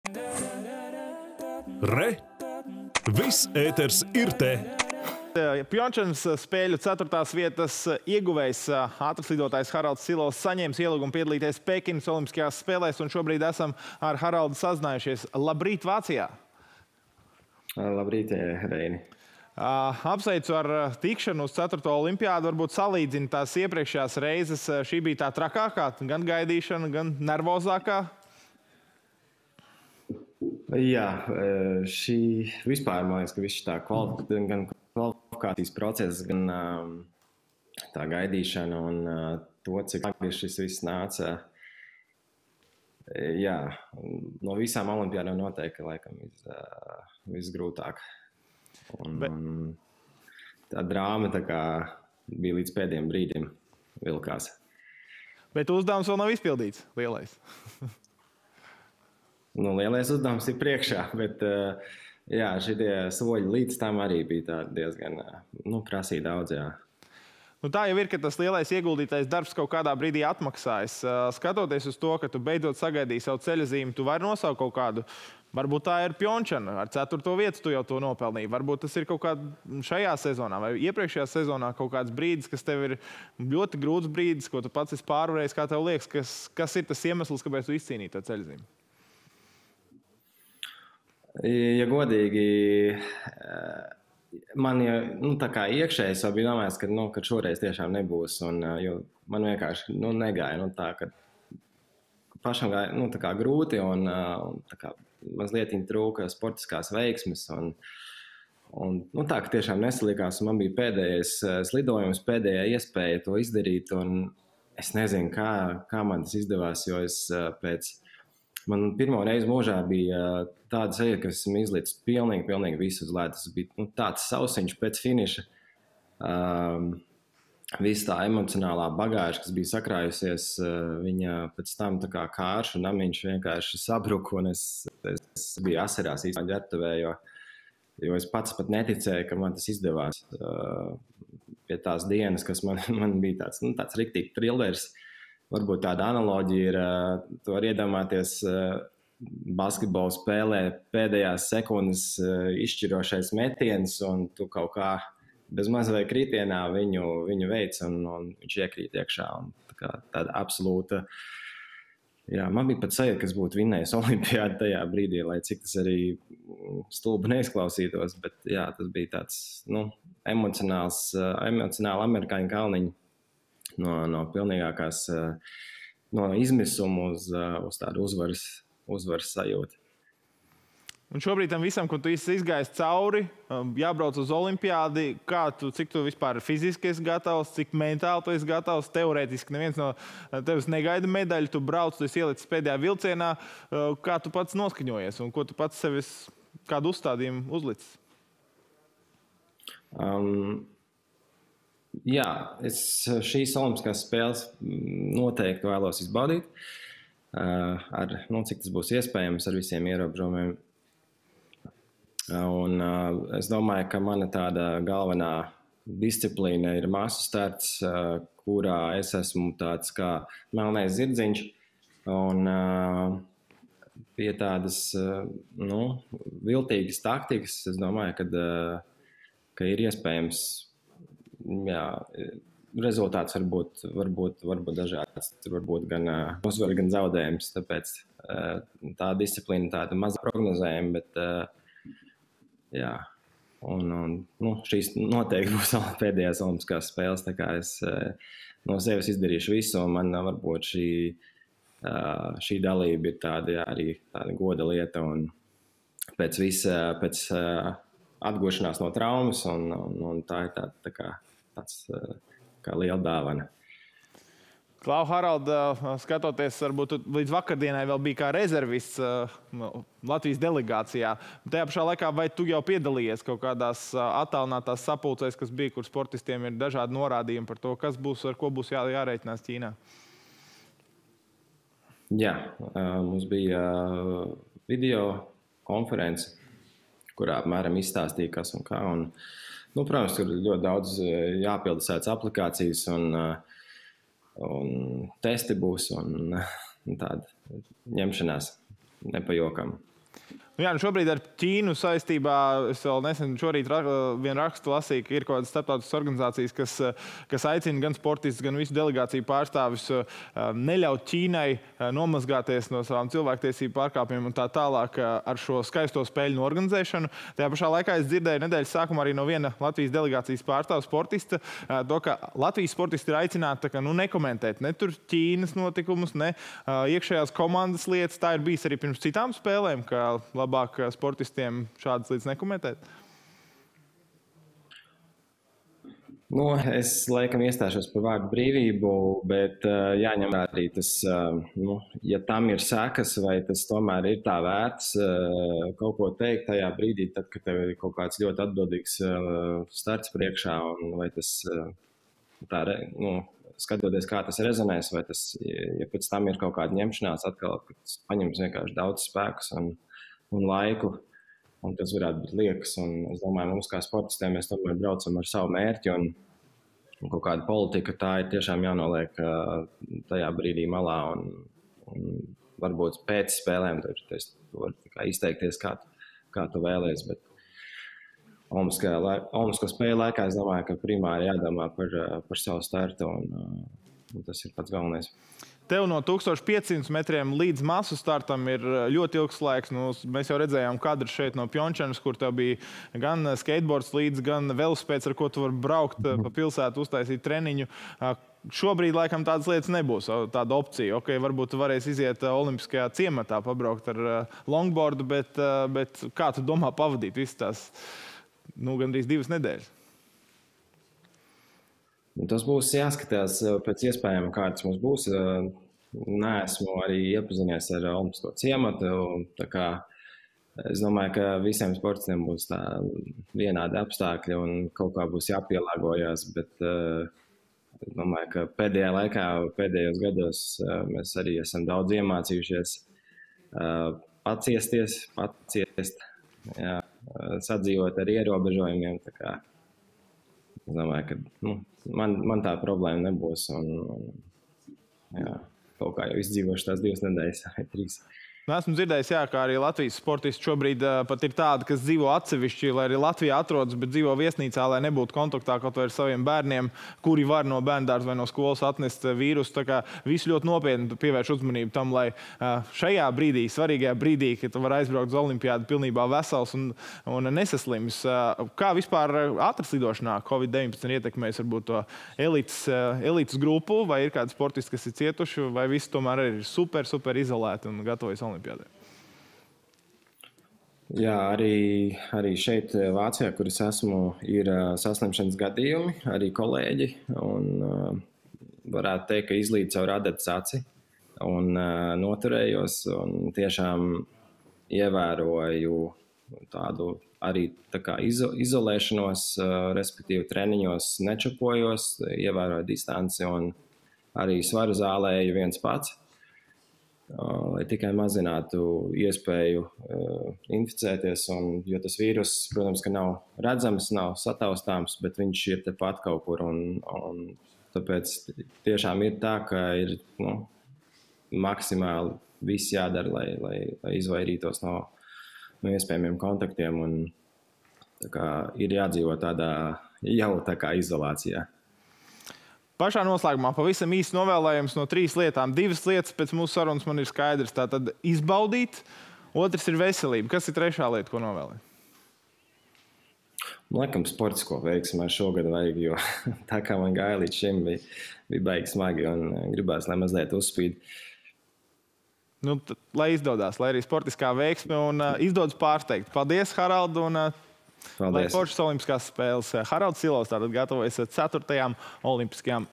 Silovs saņēmis ielūgumu piedalīties Pekinas olimpiskajās spēlēs
Reiņa Ošenieka saruna ar ātrslidotāju Haraldu Silovu.